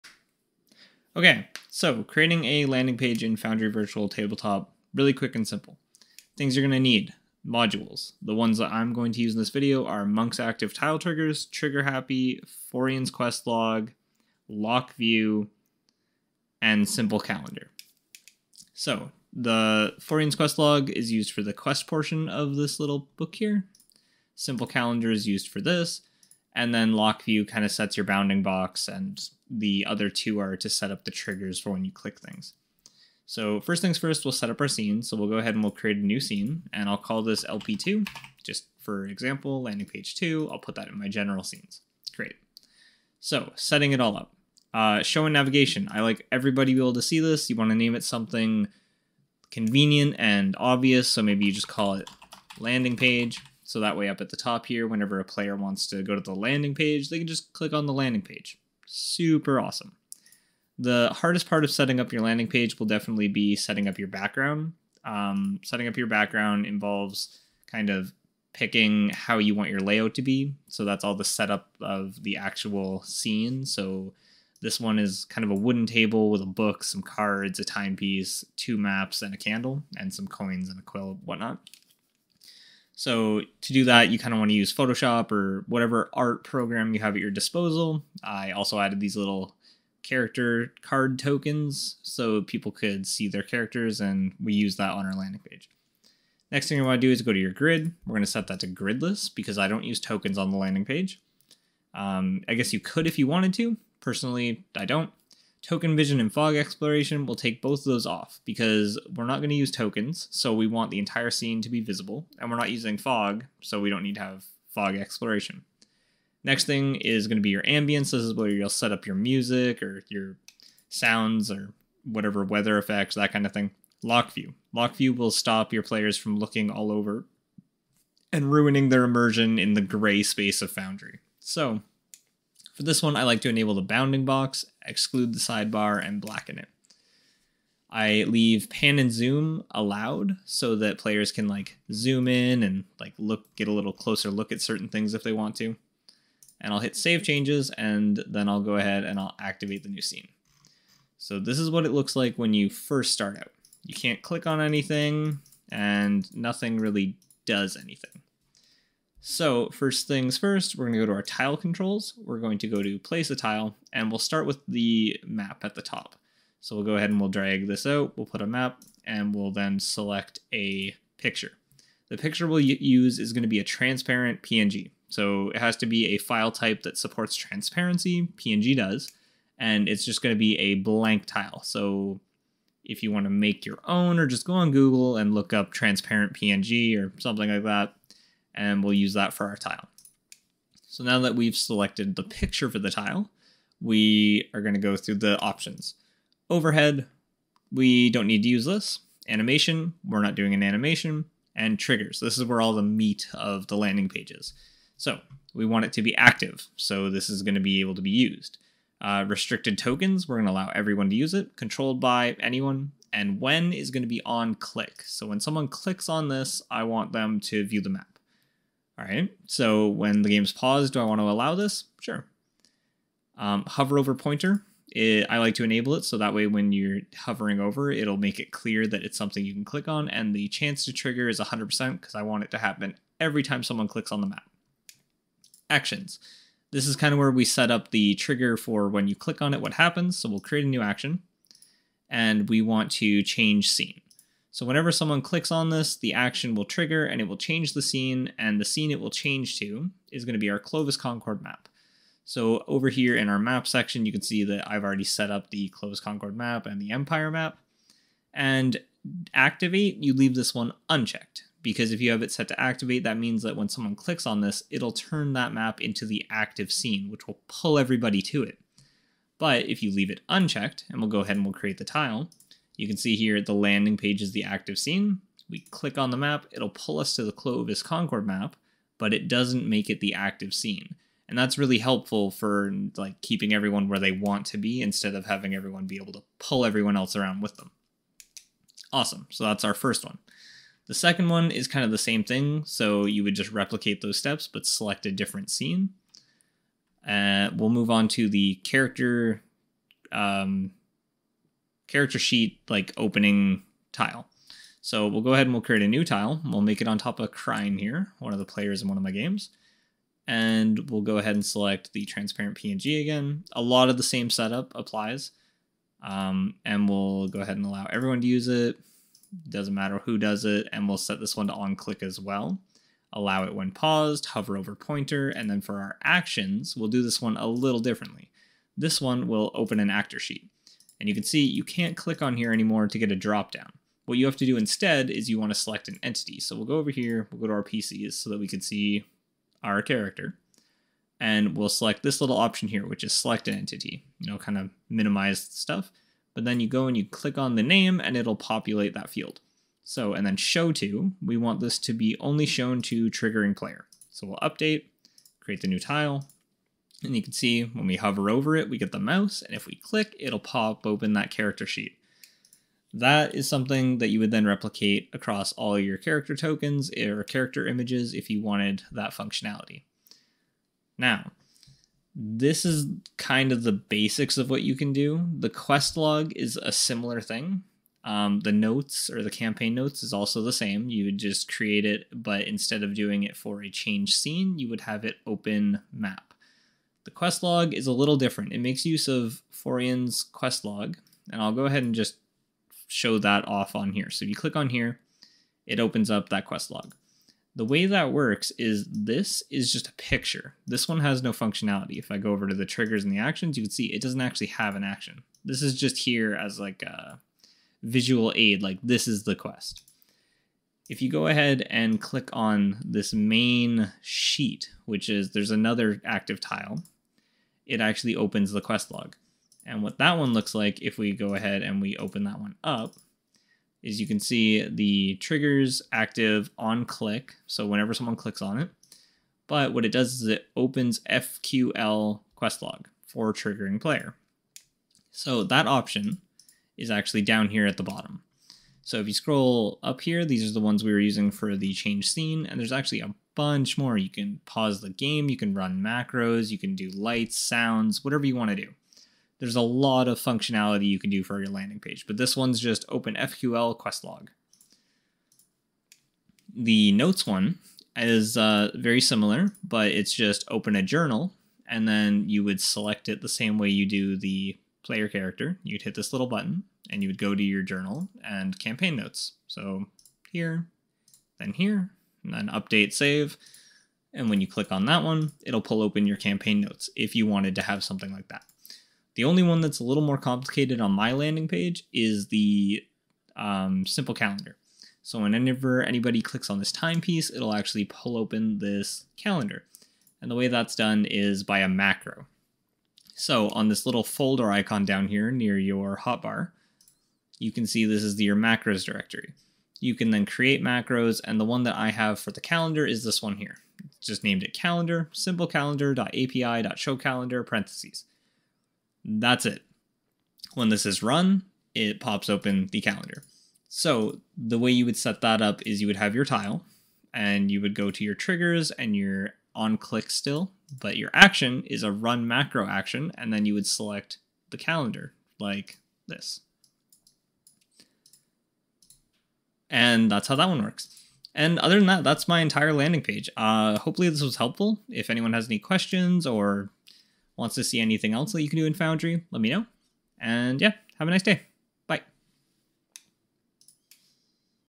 Creating a Landing Page in Foundry VTT Simplified!
A quick video on creating a landing page for Foundry VTT! If you have any trouble please let me know and I'll do my best to help out in the comments!

Modules used/required:
Monk's Active Tile Triggers (required)
Forien's Quest Log
Trigger Happer (required)
Simple Calendar
Lock View